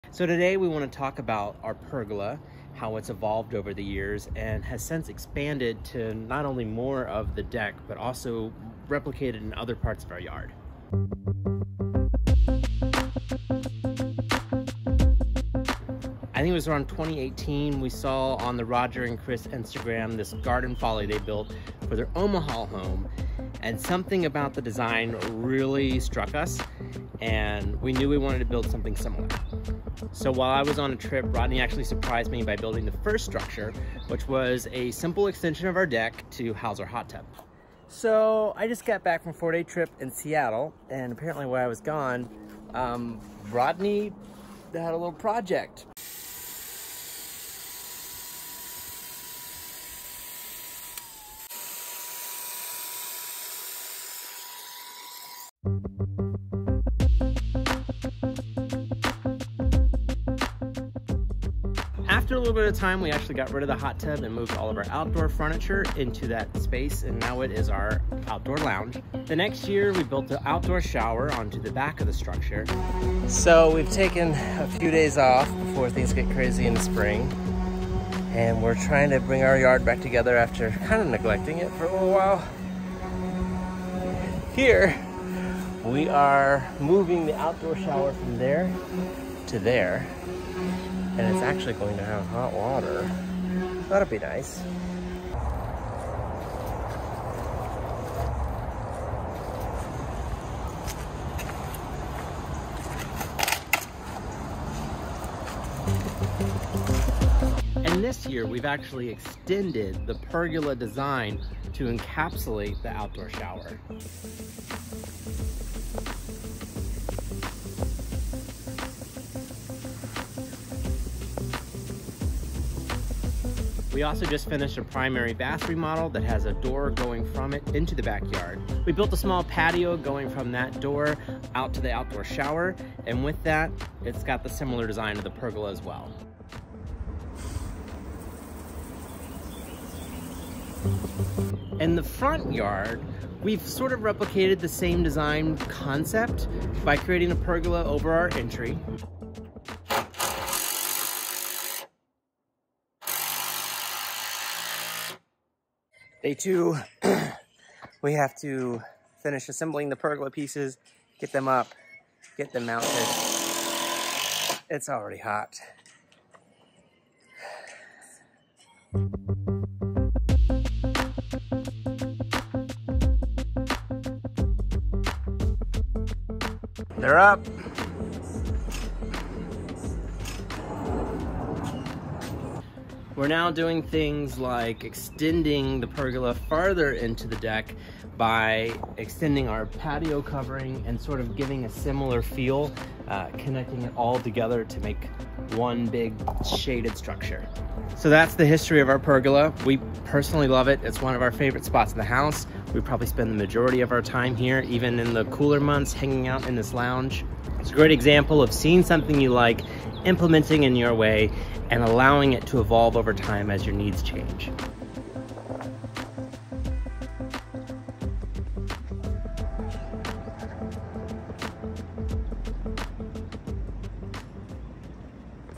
Our DIY Pergola Glow-Up: Before vs After
Dive into the journey of how our backyard transformed with the addition of a custom pergola, inspired by Roger and Chris's garden folly. Discover how what started as a simple deck extension for a hot tub evolved into an outdoor lounge, a stylish outdoor shower with hot water, and finally, an expansive shaded structure that ties our outdoor spaces together. From spontaneous projects to planned expansions, see how our pergola became the heart of our outdoor living space, enhancing both functionality and aesthetic appeal.

Whether you're looking for inspiration for your own backyard or just love a good transformation story, join us as we share the evolution of our pergola and how it's become our favorite spot at home, year-round.